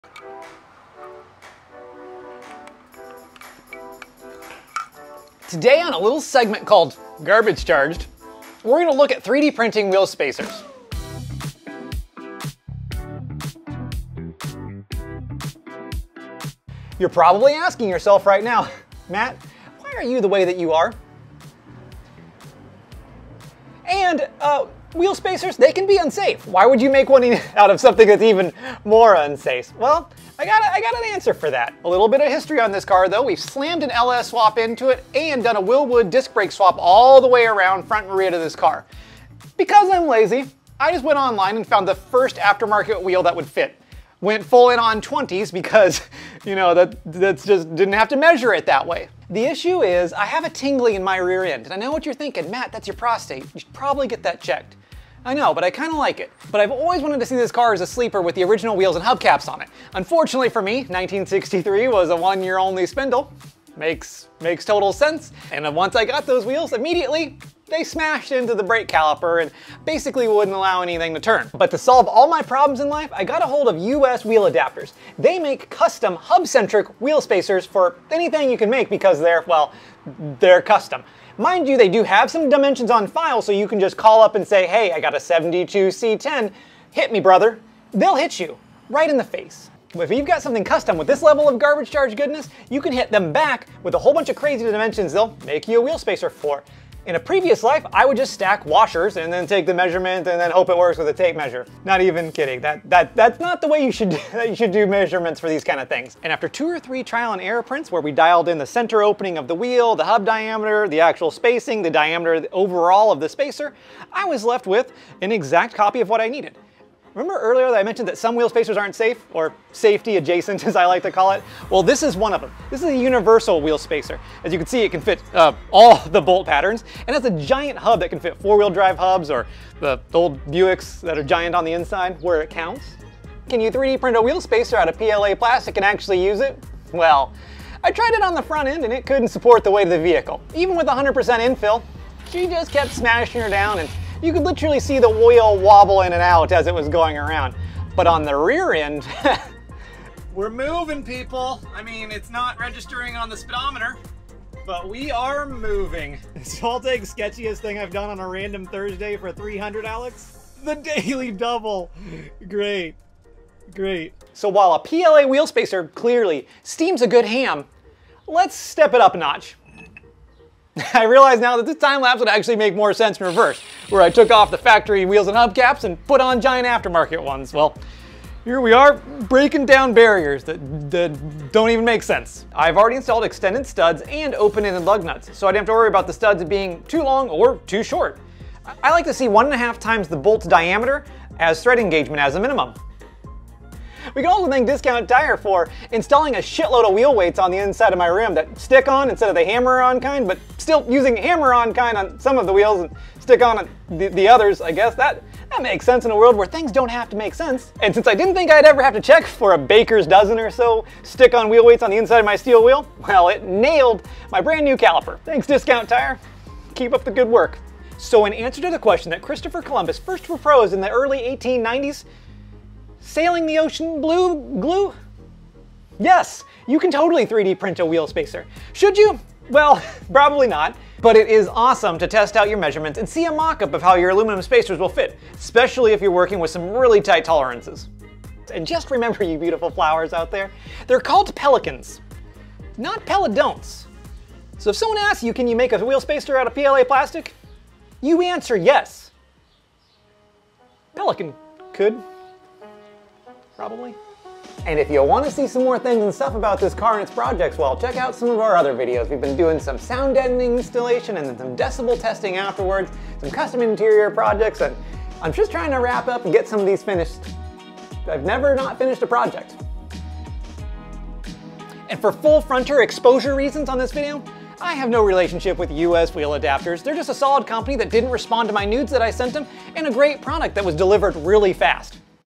3D Printing A Wheel Spacer?? US Wheel Adapter Spacer Install
Maybe a PLA wheels spacer is just better for testing...

While I love the current US Mag Rambler wheels on our 1963 LS swap project, I've always wanted to see what it would look like with the factory steel wheels and hub caps installed. Since we have converted to disk brakes and Wilwood calipers all the way around though, I couldn't just bolt the old wheels on without needing a wheel spacer. Let the fun begin!

With so many measurements to think about, I decided to put it all into CAD and 3D print a mock up of the wheel spacer I would need. After some trial and error I was left with a perfectly fit wheel spacer! But that got me thinking... Can you actually use a 3D printed spacer?? Well I printed another in Sunlu PLA with 100% infill on our Creality Ender 3 Pro. I tossed it on the front wheel and set the car back on the ground. I came out the next morning and jacked it up to see it had deformed, giving the car a couple degrees of camber. The spacer had "squeezed" together a couple of millimeters.

Not to give up that quickly - I printed a fresh PLA wheel spacer and tossed it on the rear. My thinking is  a Buick is heavy, but it's less heavy in the rear. I removed the temporary universal wheel spacer and installed our PLA spacer. I let it sit overnight again and the next morning the wheel still spun true. Now that I've got all the dimensions I needed, it was time to send everything over to US Wheel Adapter to fabricate a custom set of hub centric wheels spacers.